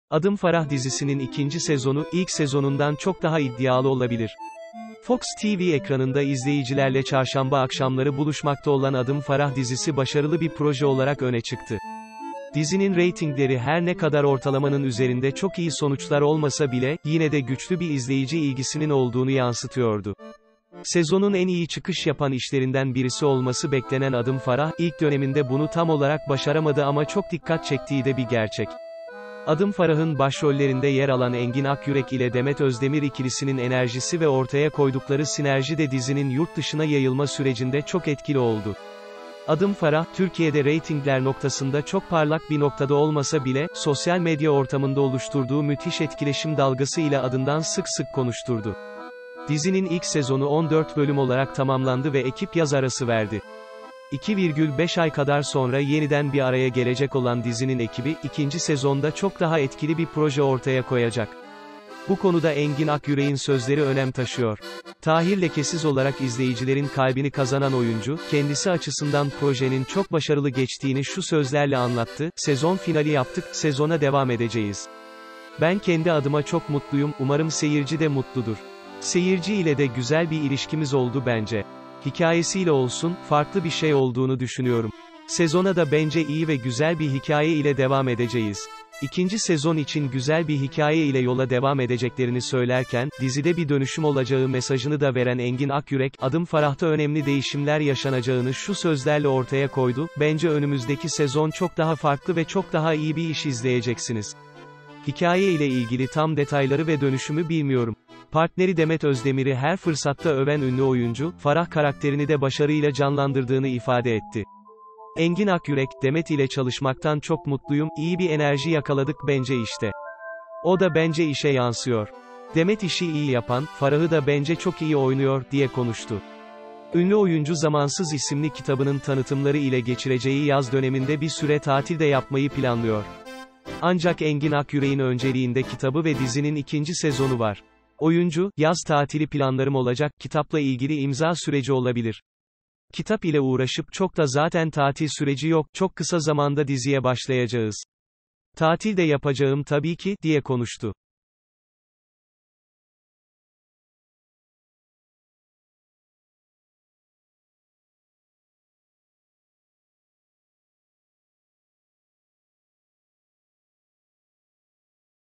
ADIM FARAH DİZİSİNİN İKİNCİ SEZONU, İLK SEZONUNDAN ÇOK DAHA İDDİALI OLABİLİR!
İran'dan kaçıp, hamile olduğunu öğrenmesiyle İstanbul'da kalmak zorunda kalan Farah; asıl mesleği olan cerrahlık yerine temizlik işçisi olarak çalışmaya başlar. Doğuştan balon hastalığı olarak bilinen PİY rahatsızlığına sahip olan oğlu Kerimşah'a hem annelik hem de doktorluk yapmayı başaran Farah'ın; oğlu ve onun tedavisi için yapamayacağı şey yoktur.Bir gece, temizlik yaptığı bir mekanda, istemeden şahit olduğu trajik olay vesilesi ile Tahir Lekesiz ile karşı karşıya gelir. Tahir; yasadışı işlerle uğraşan bir aile şirketinin tepe elemanlarından biridir ve hayatını kurtarmış, küçük yaşta onu yanına alıp yetiştirmiş patronuna sadakatle bağlı bir adamdır. Fakat o gece kendini ilk defa beklemediği bir durumda bulur. Farah susmak ya da adaleti sağlamak için konuşmak arasında bocalarken, Tahir ise hayatında ilk defa masum bir kadın ve çocuğun kaderiyle ilgili karar vermek zorundadır.